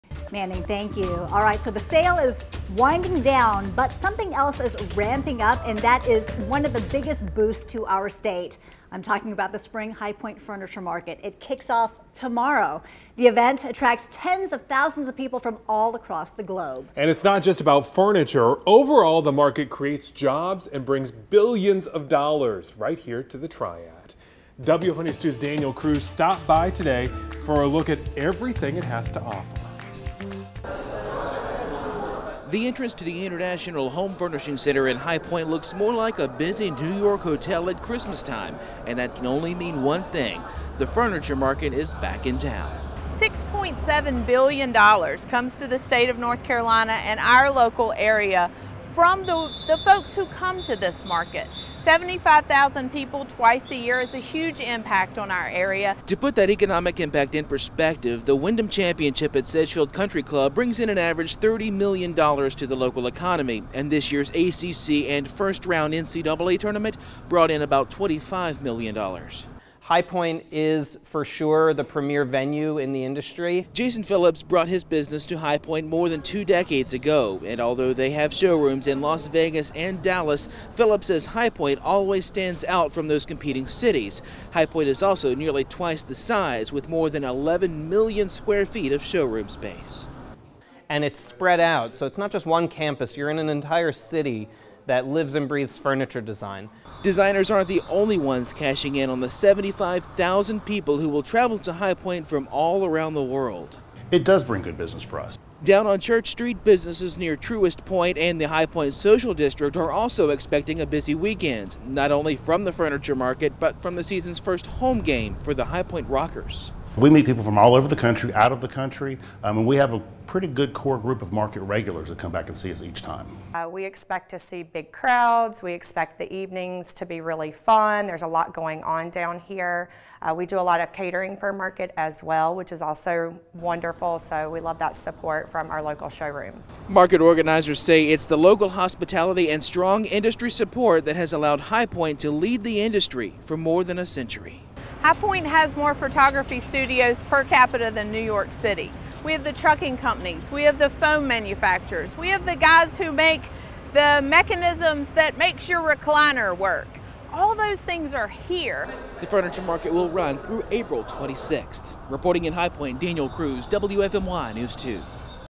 Spring High Point Furniture Market attracting worldwide vendors
Rain or shine the 2023 spring High Point Furniture market kicks off on April 22. It's one of North Carolina's largest economic events.

The crowds can only mean one thing, the Furniture Market is back in town.